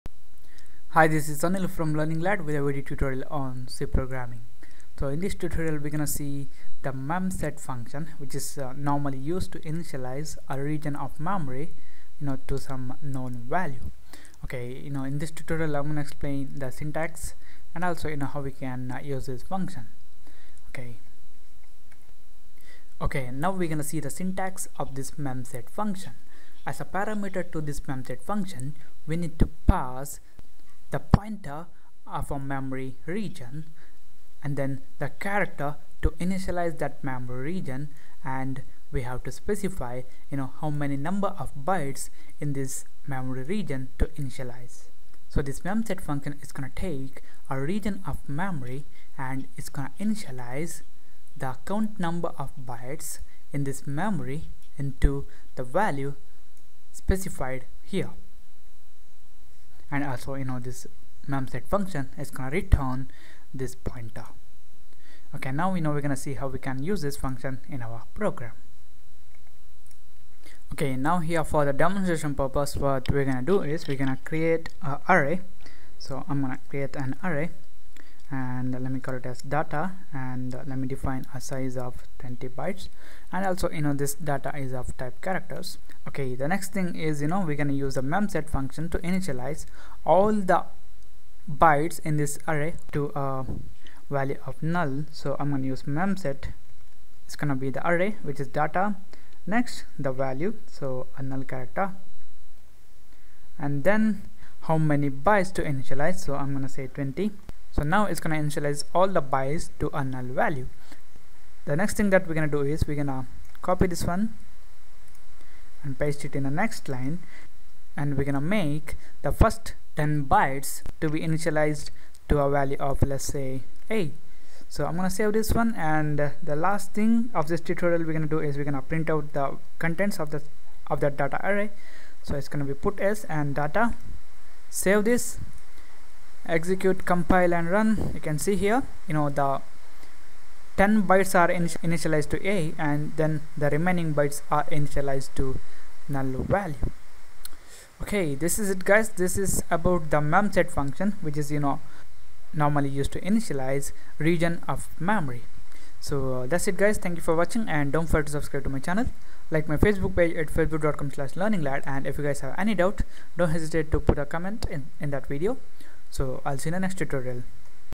memset Function in C Programming Language Video Tutorial
In this C programming language video tutorial / lecture for beginners video series, you will learn about the memset() function in c programming with its syntax in detail with example.

You will learn what is the use of memset function, what is the syntax of it and also how to use it to initialize the memory.

The tutorial begins with the syntax and then explains what this function is gonna do in detail. Then it explains the usage it to initialize the memory to some value.